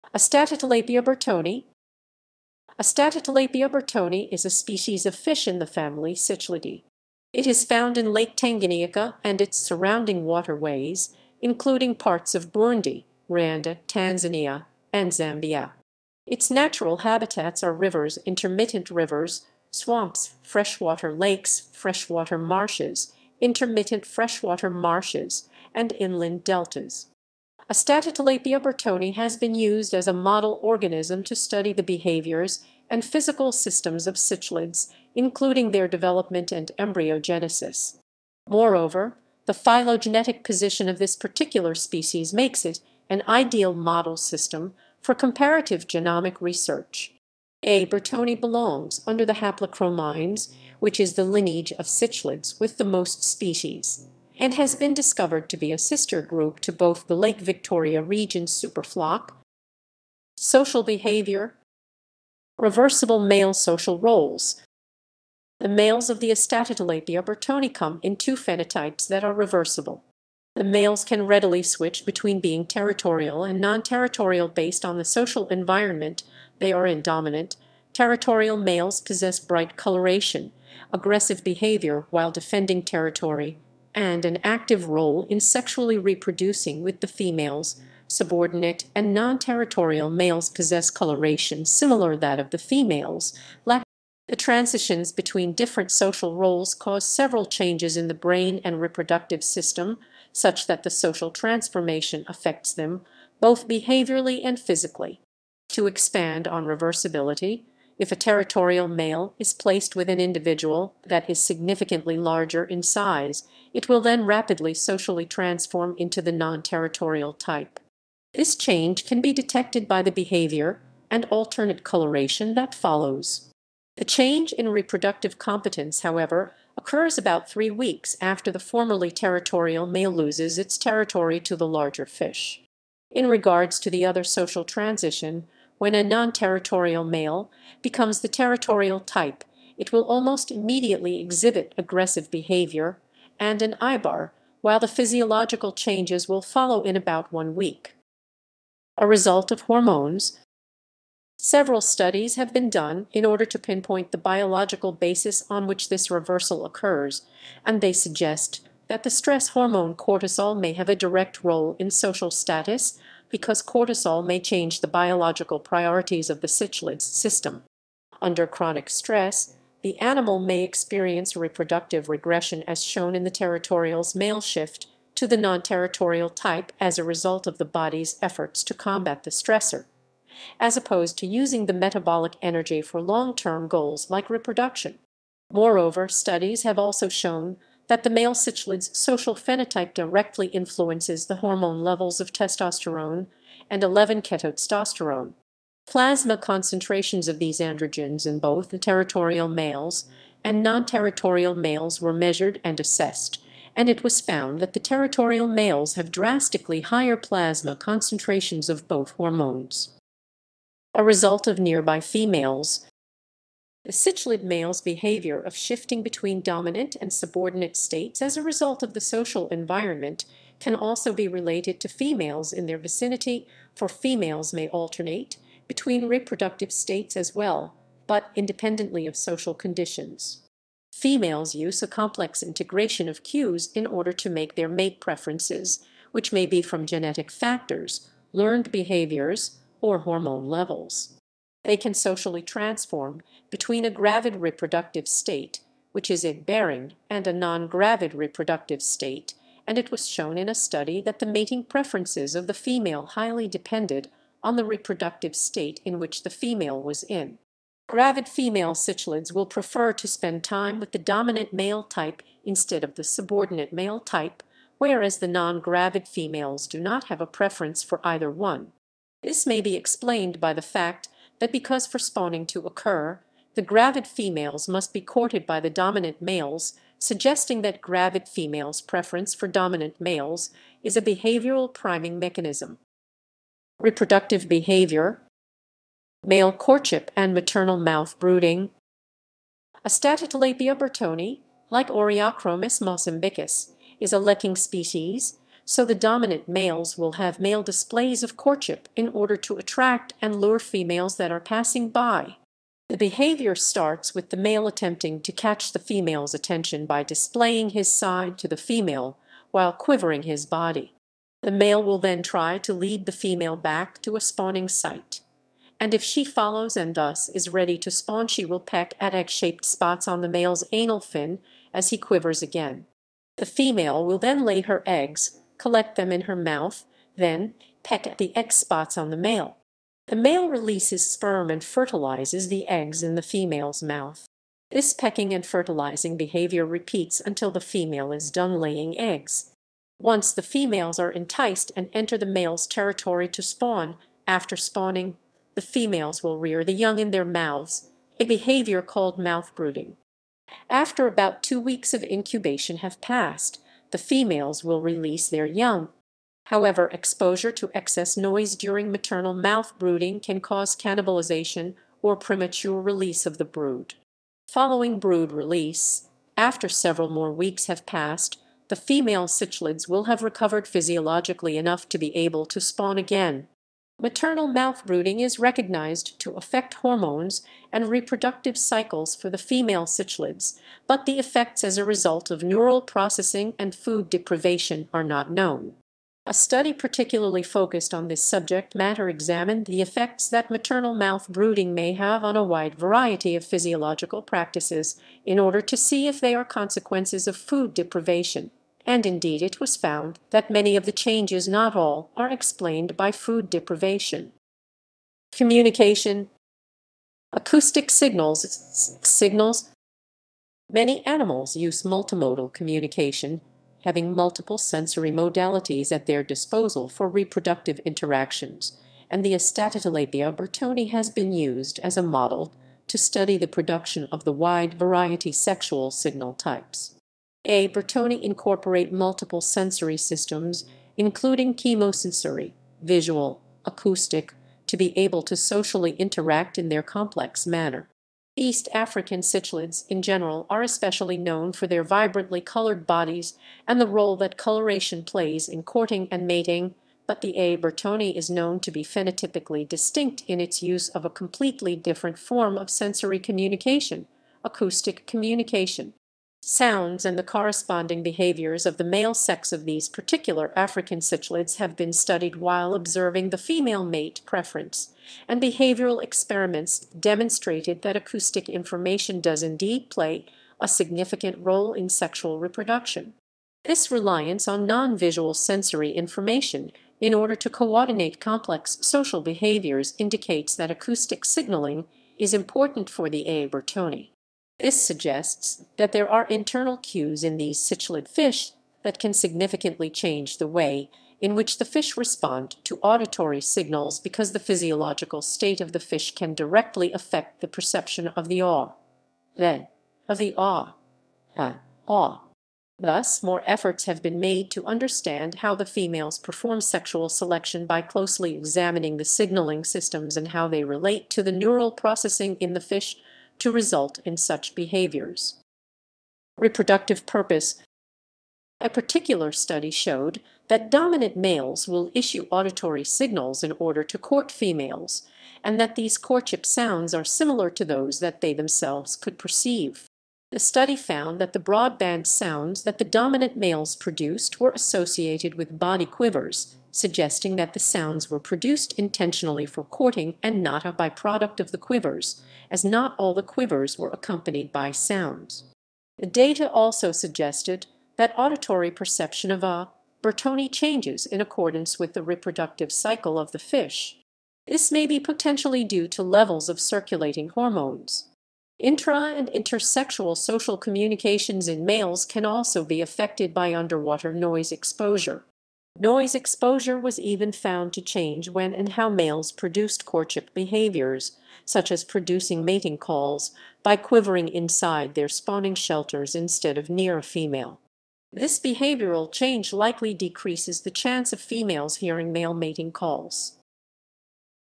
Astatotilapia burtoni
Astatotilapia burtoni is a species of fish in the family Cichlidae.
It is found in Lake Tanganyika and its surrounding waterways, including parts of Burundi, Rwanda, Tanzania, and Zambia.
Its natural habitats are rivers, intermittent rivers, swamps, freshwater lakes, freshwater marshes, intermittent freshwater marshes, and inland deltas.
Astatotilapia burtoni has been used as a model organism to study the behaviors and physical systems of cichlids, including their development and embryogenesis. Moreover, the phylogenetic position of this particular species makes it an ideal model system for comparative genomic research. A. burtoni belongs under the haplochromines, which is the lineage of cichlids with the most species, and has been discovered to be a sister group to both the Lake Victoria region superflock (which has about 600 species) and the species flock of Lake Malawi (which has about 1,000 species).